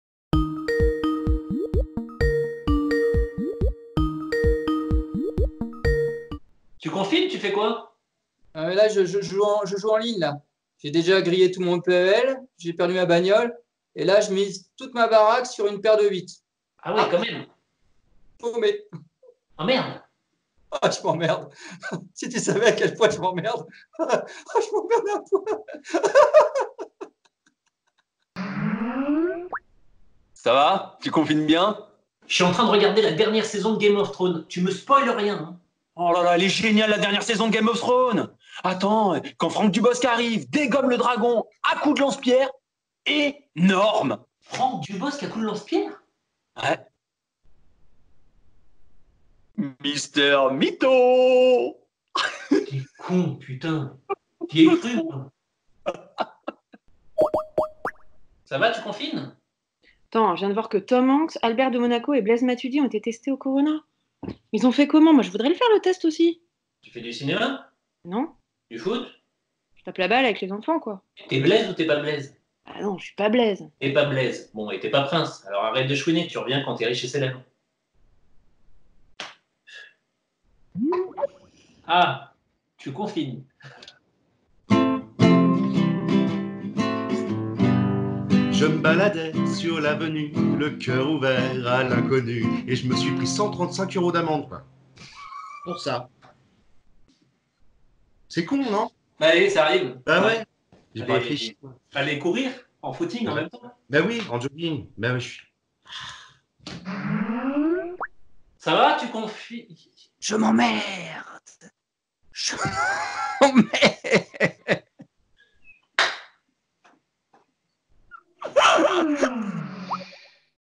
TU CONFINES ? EP8
TU CONFINES ?
8ème épisode !!!

On chantonne, on papote, on confine
Tous les jours 21H !
Avec : Jean-Marc Magnoni, Yvon Carpier, Laura Guillemin​, Antoine Réjasse,​ eta ospetsua Seb Séga​